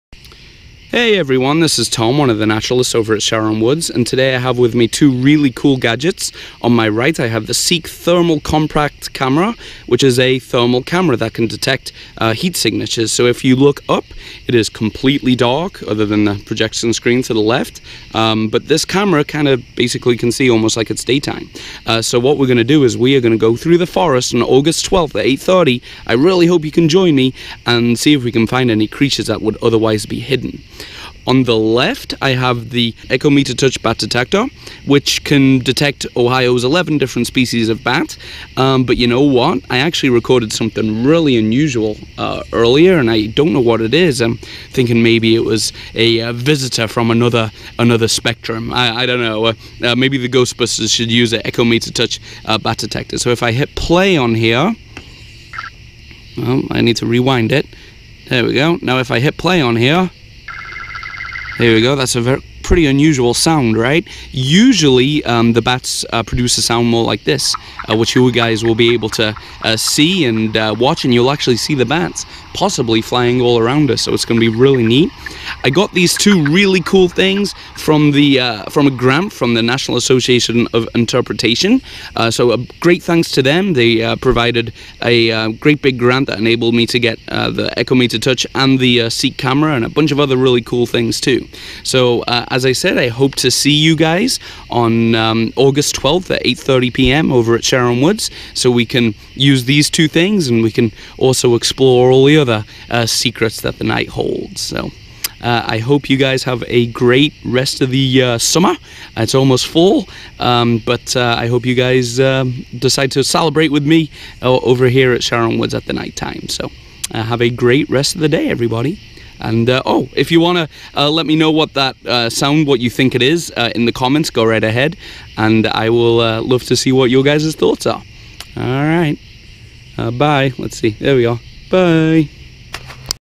Nocturnal Adventures
Thanks in part to National Association for Interpretation Region 4,​  one of our naturalists has been studying nocturnal (and possible otherworldly) creatures. Join him Saturday, August 12, at 08:30 PM to look for the creatures of the night!